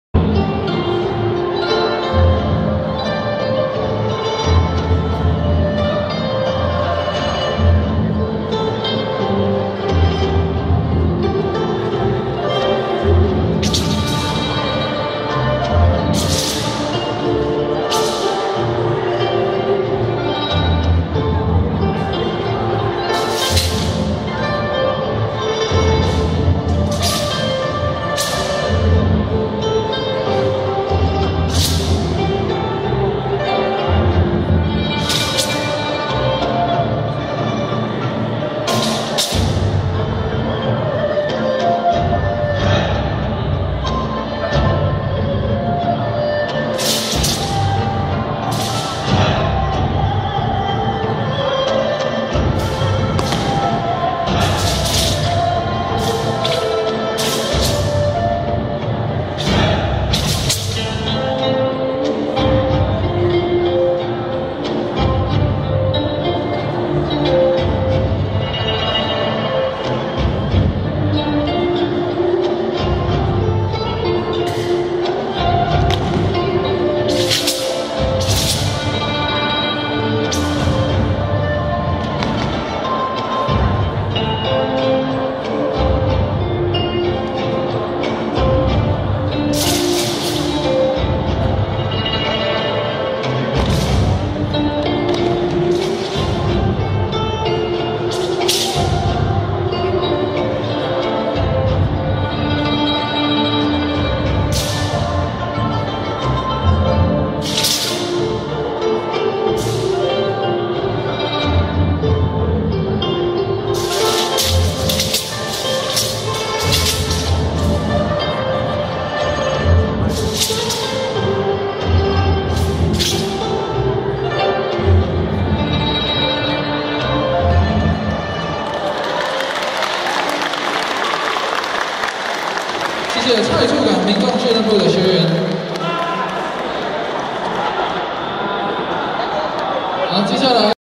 陈式三十六刀 蔡厝港民众倶乐部 指导老师 : 王进才老师 义顺东太极观摩会 4 Aug 2013
陈式三十六刀 蔡厝港民众倶乐部
指导老师 : 王进才老师
义顺东太极观摩会 4 Aug 2013